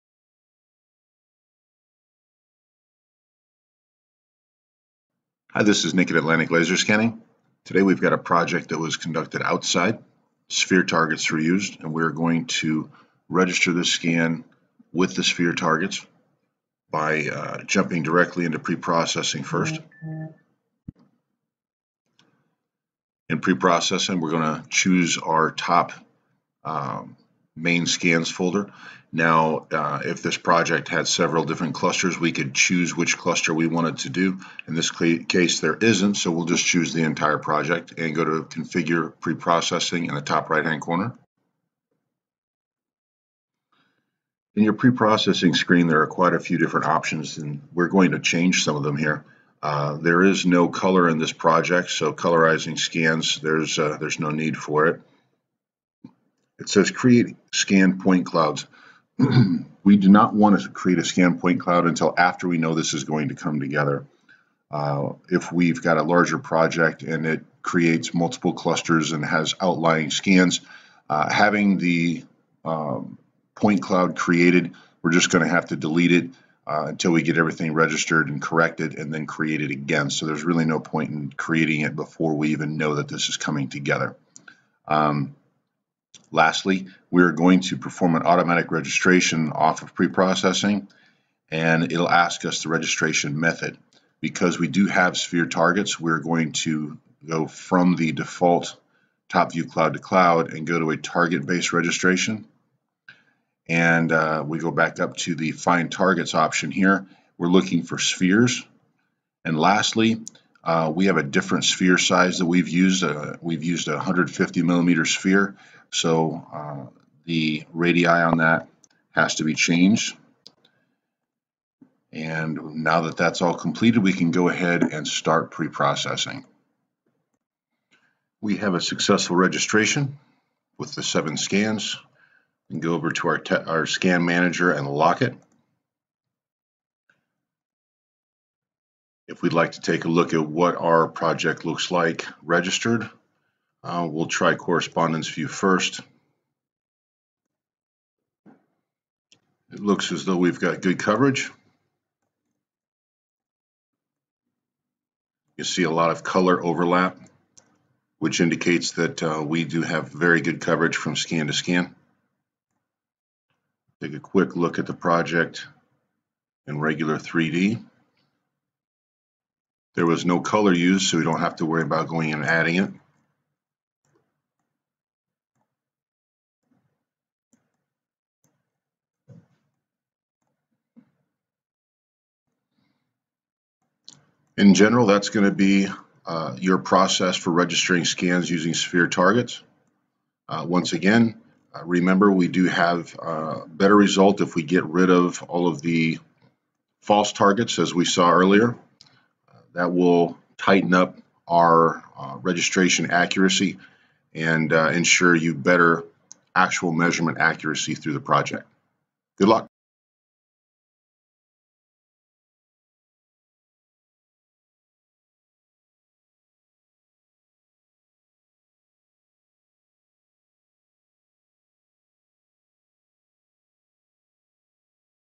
Atlantic Laser Scanning Tutorial FARO SCENE Registration With Sphere Targets Workflow
This Atlantic Laser Scanning tutorial used point cloud data from a FARO Focus 3D scanner, importing it into FARO SCENE and registering it.  Because sphere targets were used in the field by the scan tech the spheres are used by the SCENE software as reference points during registration.  Atlantic Laser Scanning, a FARO 3D Laser Scanner Rental and Training Company
Atlantic Laser Scanning, a FARO 3D Laser Scanner Rental and Training Company